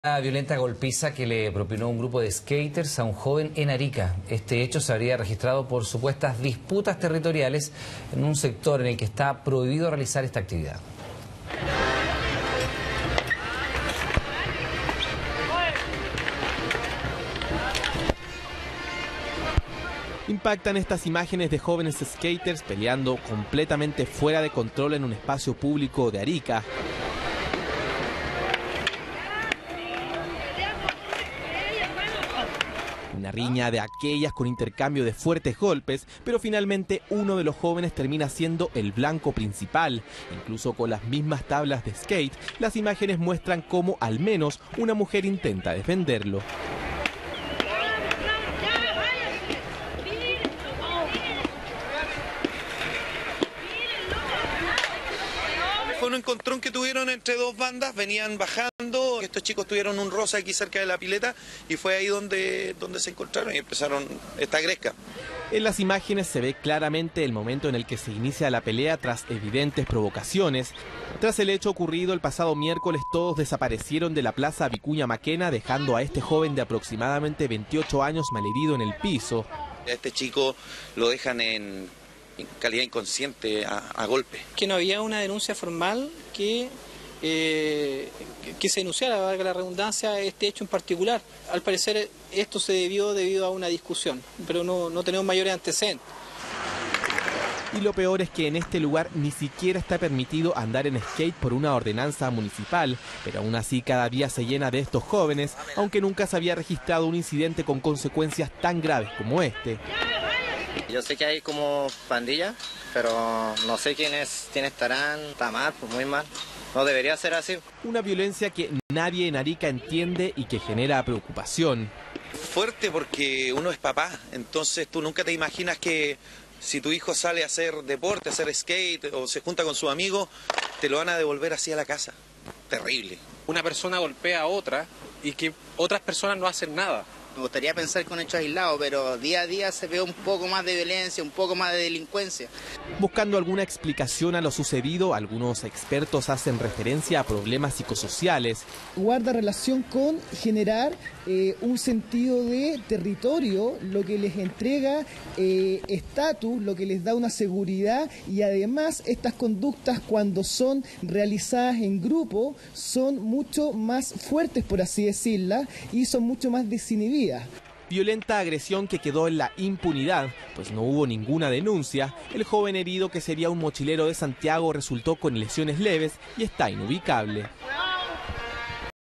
Crudo video muestra brutal golpiza a skaters en Arica
Una violenta golpiza se vivió en Arica, donde un joven skaters fue atacado por un grupo de jóvenes. El hecho se habría registrado por disputas territoriales en un sector en el que está prohibido realizar este deporte.

En la impactante filmación se aprecia cómo alrededor cuatro jóvenes le pegan salvajemente a otro en plena plaza Vicuña Mackenna y a vista de varias personas que anonadadas no encuentran respuesta a este violento suceso.